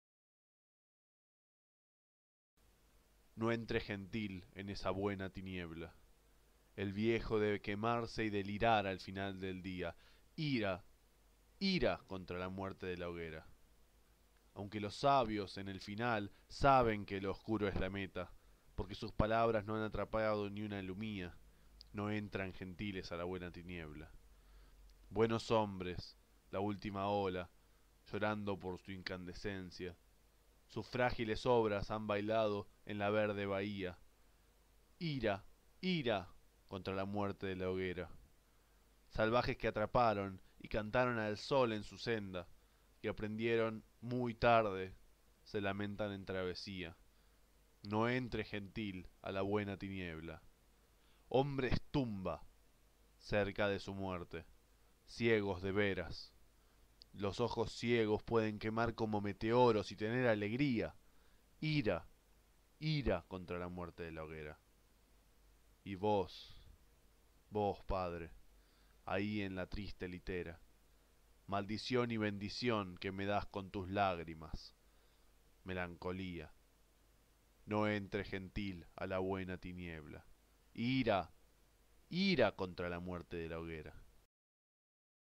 No entres gentil en esa buena tiniebla - Dylan Thomas
Traducción y narración por Joaquín Rodriguez:

No entres gentil en esa buena tiniebla

el viejo debe quemarse y delirar al final del día

ira, ira contra la muerte de la hoguera

Aunque los sabios, en el final, saben que lo oscuro es la meta

porque sus palabras no han atrapado ni una lumía

no entran gentiles a la buena tiniebla

Buenos hombres, la ultima ola, llorando por su incandescencia

sus frágiles obras han bailado en la verde bahía

Salvajes que atraparon y cantaron al sol en su senda

y aprendieron, muy tarde, se lamentan en travesía

no entres gentil a la buena tiniebla

Hombres-tumba, cerca de su muerte, ciegos de veras

los ojos ciegos pueden quemar como meteoros y tener alegría

Vos, padre, ahí en la triste litera

maldición y bendición, que me das con tus lágrimas. Melancolía.